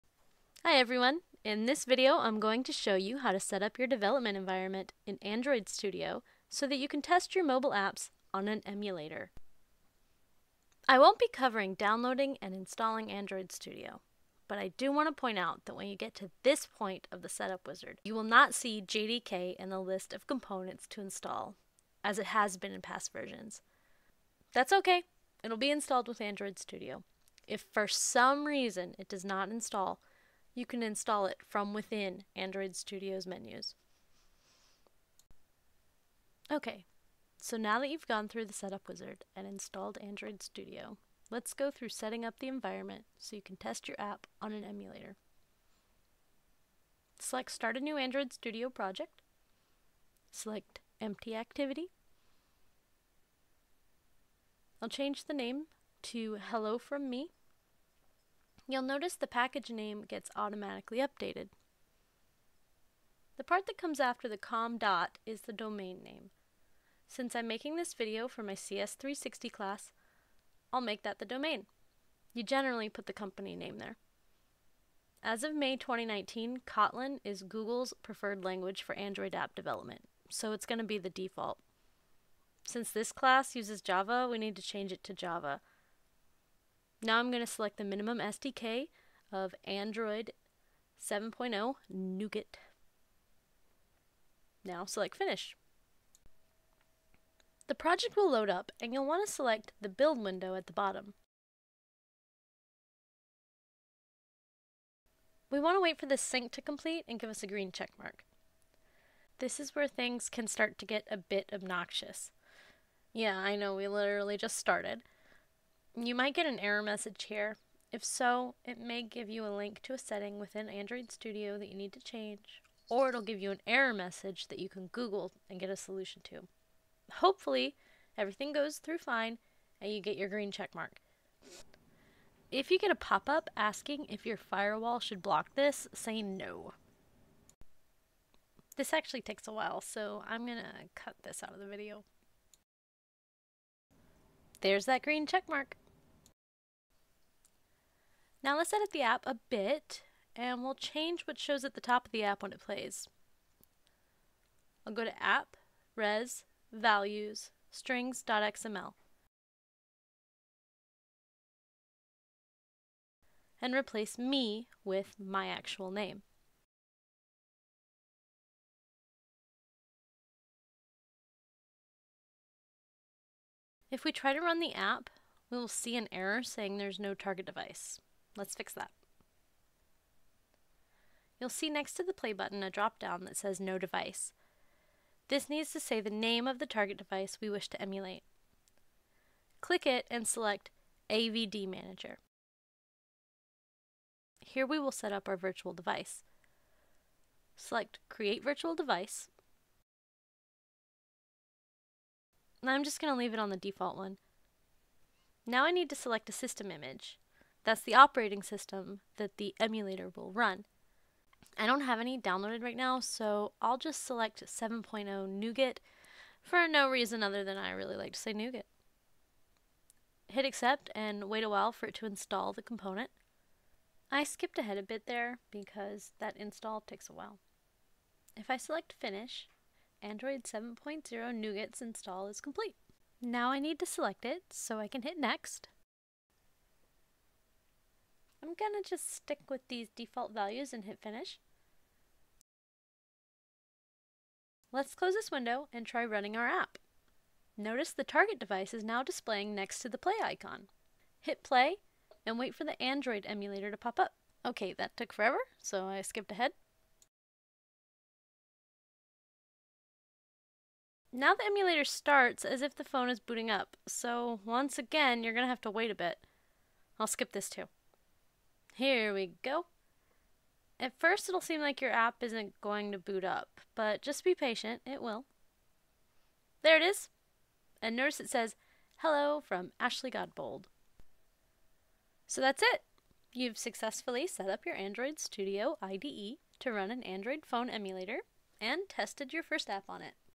Setting up Android Studio's Environment for Testing on a Mobile Emulator (Windows 10)
This video demonstrates setting up the Android Studio Environment for mobile app development with an emulator.

To be used by CS360 for Assignment 1-2.